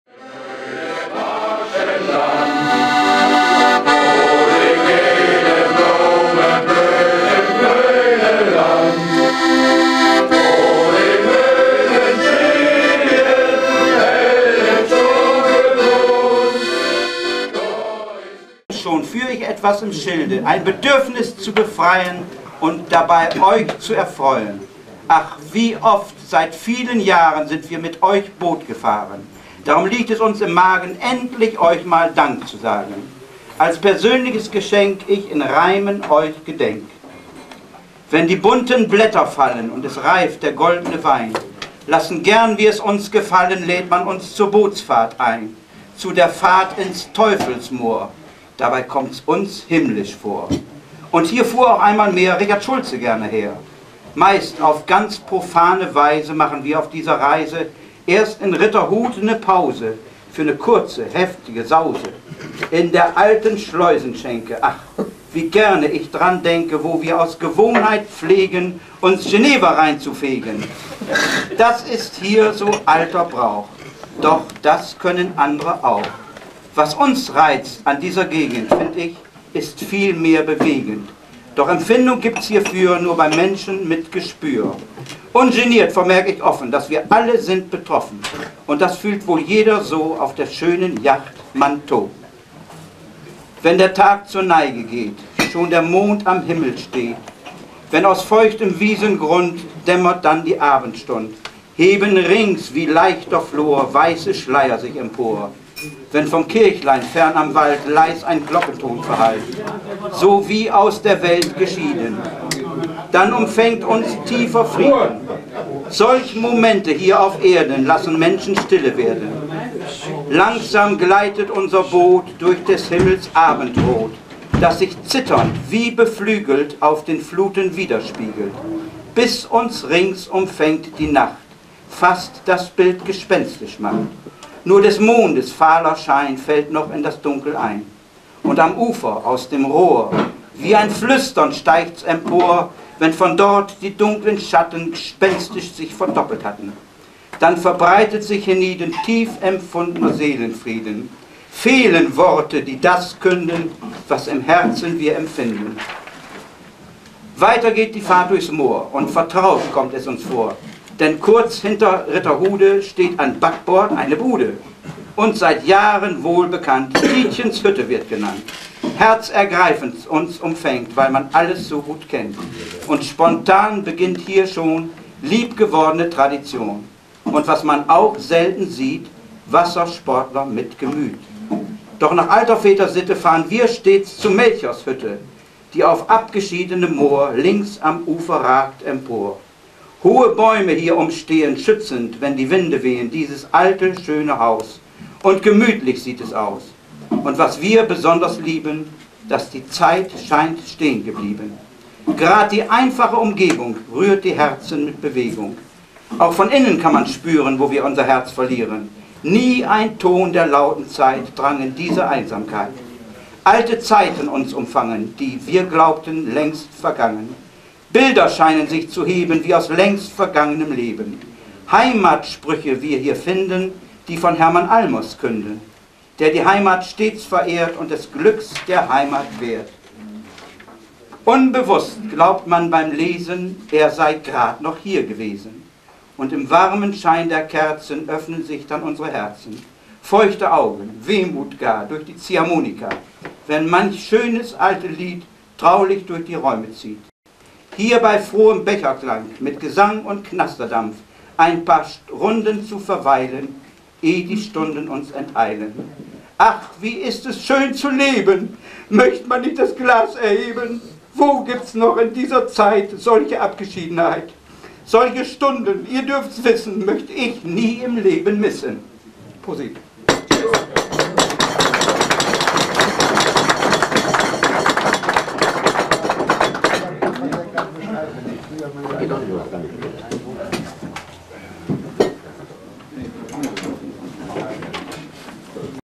OYC Gemeinschafts-Fahrten ins Teufelsmoor – Gedicht von Peter Franssen
Viele Jahre, bis zum Befahrensverbot auf der Hamme, fuhren Schiffe der Oldenburger Yacht-Club als Gemeinschaftsfahrt jedes Jahr im Oktober zu Tietjens- und Melchors Hütte ins Teufelsmoor. Peter Franssen war viele Jahre Gast auf „Man To“ von Hansi Mönnich. Schon von Krankheit gezeichnet hat Peter Franssen als Dank ein Gedicht verfasst und zu später Stunde beim 10. Nicolaussingen 1996 vorgetragen. Einige Jahre später ist Peter Franssen verstorben. Vielleicht ist seine Betrachtungsweise von Natur und Miteinander angesichts der heutigen Corona-Krise nicht ganz falsch.